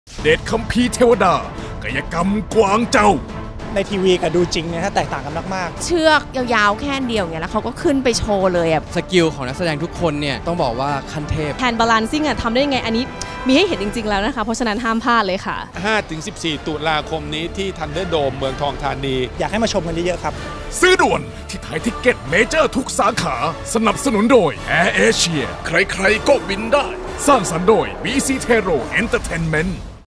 เดชคัมภีร์เทวดา กายกรรมกวางเจา
เหล่าเซเลบ-ดารา ต่างทึ่ง! เมื่อได้เห็นความสามารถของเหล่านักแสดง เดชคัมภีร์เทวดา กายกรรมกวางเจา แบบใกล้ชิด พร้อมย้ำ! ห้ามพลาด!!! งานนี้ต้องมาพิสูจน์ด้วยตาของคุณเอง 5-14 ต.ค.นี้ ที่ ธันเดอร์โดม เมืองทองธานี บัตรเข้าชมราคา 700 / 900 / 1,200 และ 1,500 บาท มีจำหน่ายที่ไทยทิคเก็ตเมเจอร์ทุกสาขา โทร. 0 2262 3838 และ